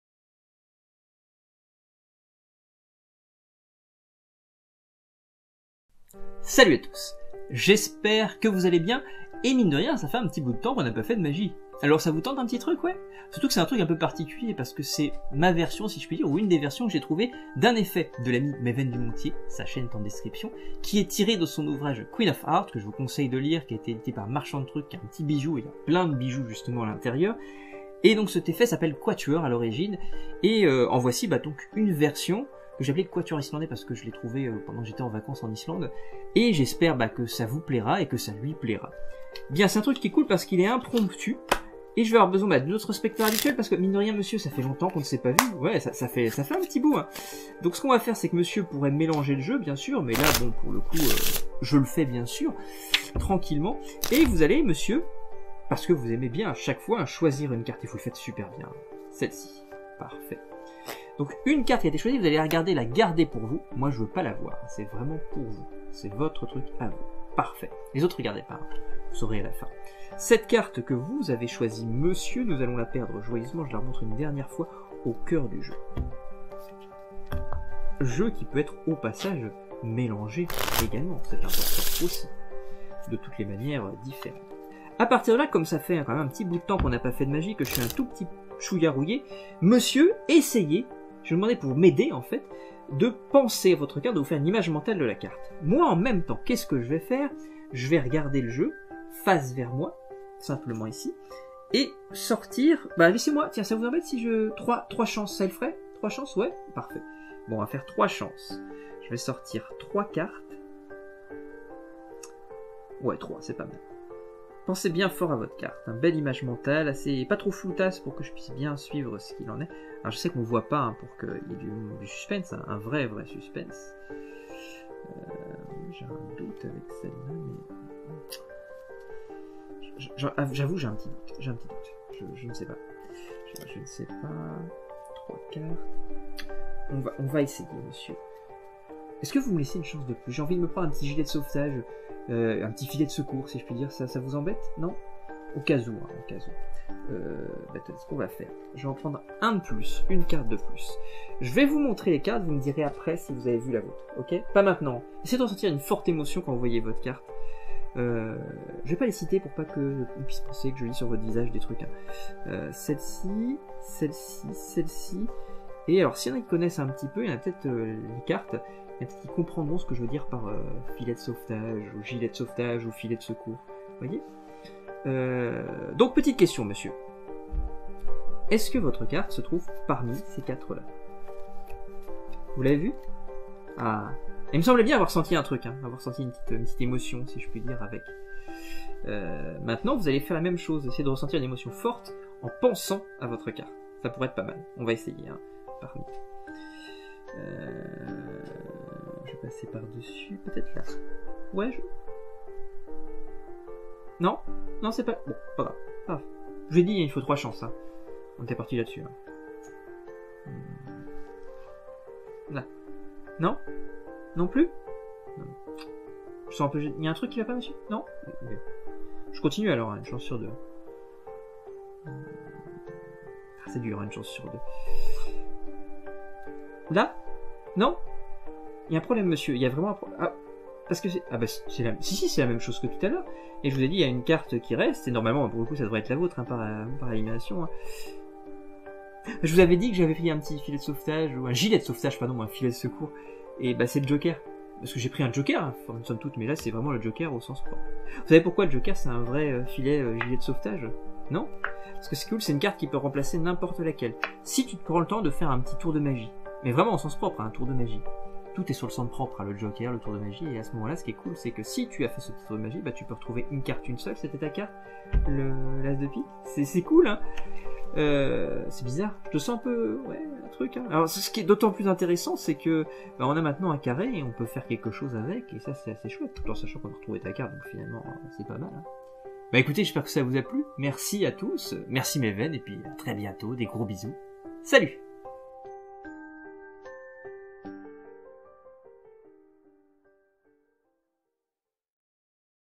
[Magie] Inspiration#38 Quatuor Islandais
Retour de la magie avec une routine construite sur une idée géniale de l'ami @MevenDumontier , Quatuor, un bijou qu'il a pu publier sur sa chaîne mais également dans son passionnant ouvrage "Queen of Art", dont je vous livre une version personnelle islandaise (trouvée en Islande j'entends ;) ) !

Des bisous !